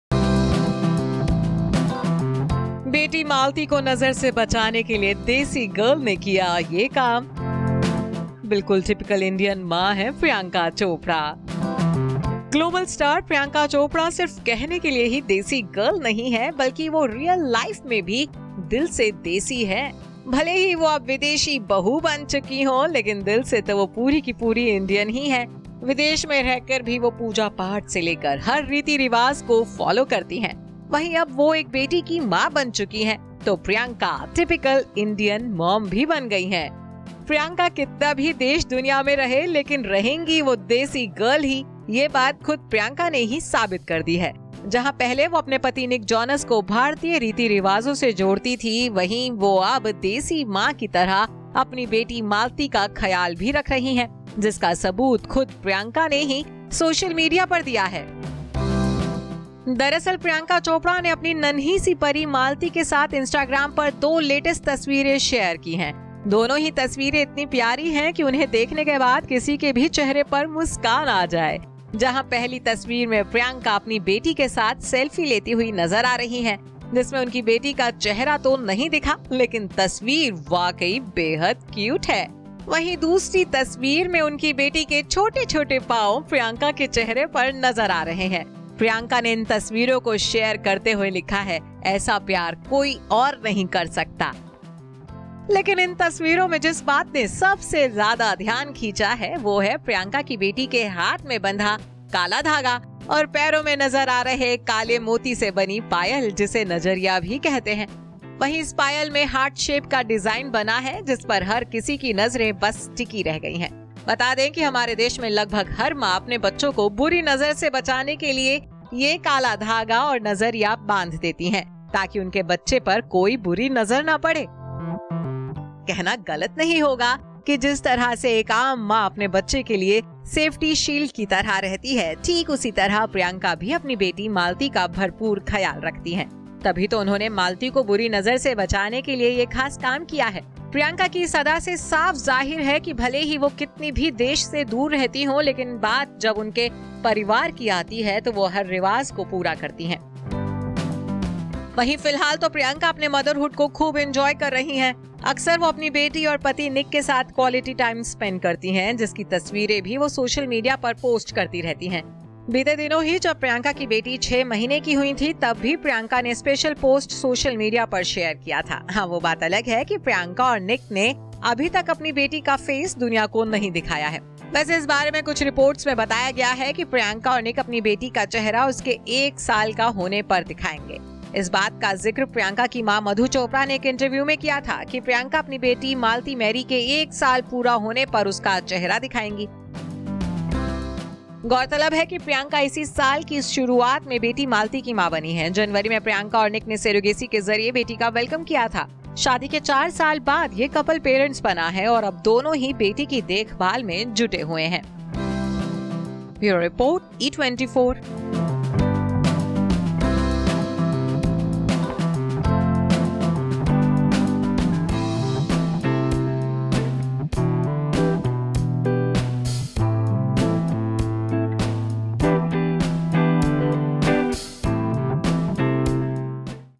अमेरिका में रहते हुए भी एकदम देसी हैं Priyanka Chopra, अपनी लाड़ली को यूं बुरी नजर से बचाती हैं
प्रियंका कितना भी देश-दुनिया में रहें लेकिन रहेंगी वो देसी गर्ल ही ये बात खुद प्रियंका ने ही साबित कर दी है। जहां पहले वो अपने पति निक जोनस को भारतीय रीति-रिवाजों से जोड़ती थीं वहीं अब वो देसी मां की तरह अपनी बेटी मालती का ख्याल भी रख रही हैं। जिसका सबूत खुद प्रियंका ने ही सोशल मीडिया पर दिया है।